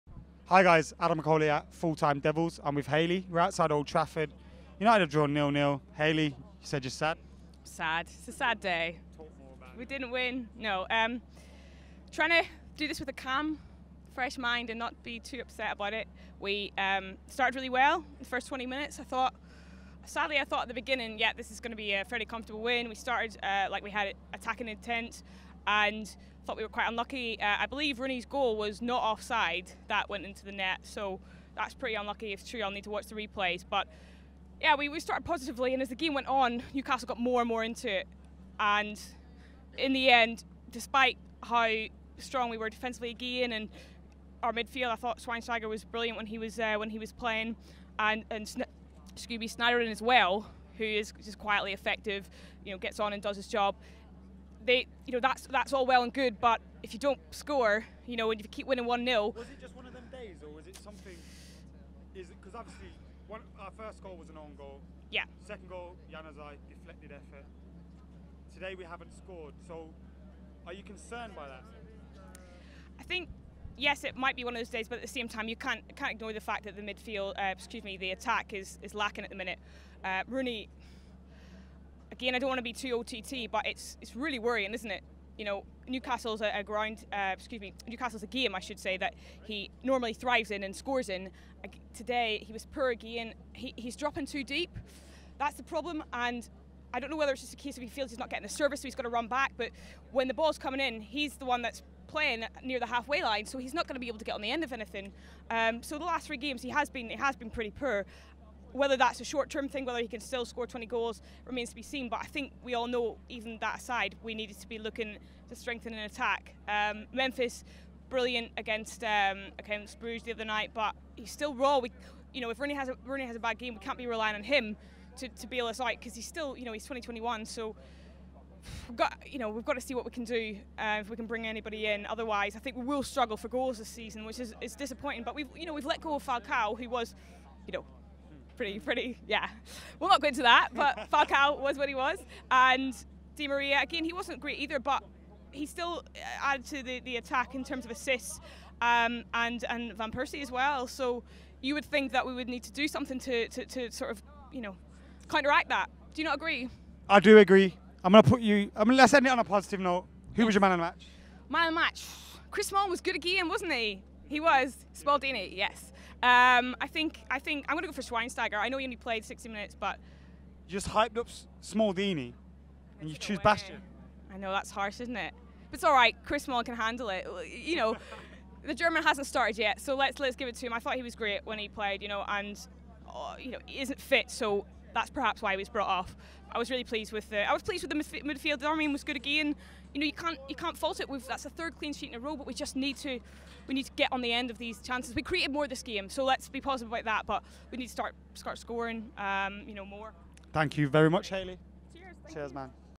Rooney Wasn't Offside | Manchester United 0-0 Newcastle United | FANCAM
Hayley doesn't think Rooney was offside for what should have been the opener in Manchester United 0-0 Newcastle United, was it? Did that change the game for us?